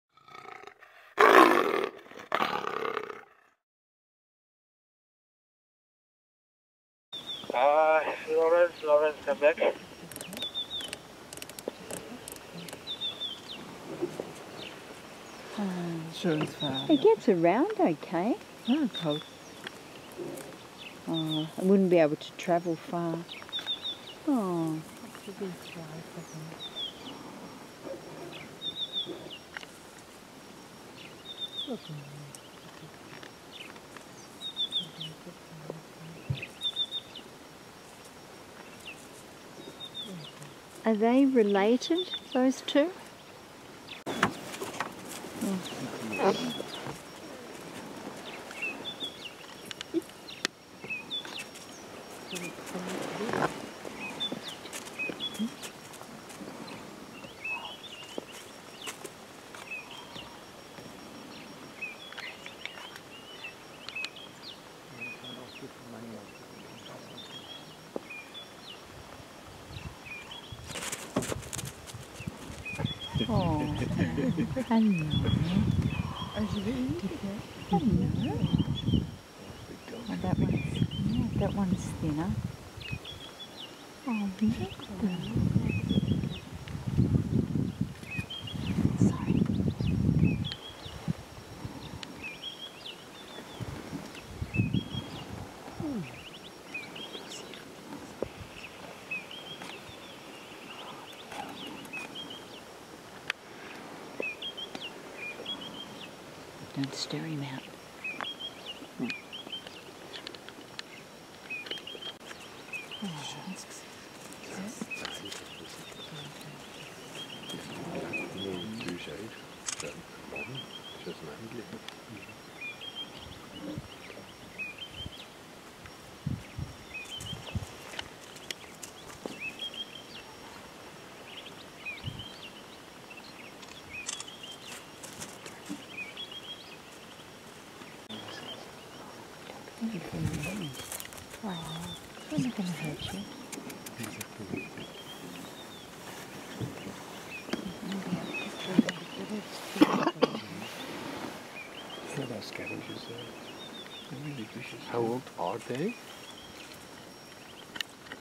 Sticknyawo the amazing 3-legged hyena
Stcknyawo playing alongside the Pondoro safaricruiser on the25th of July 2015.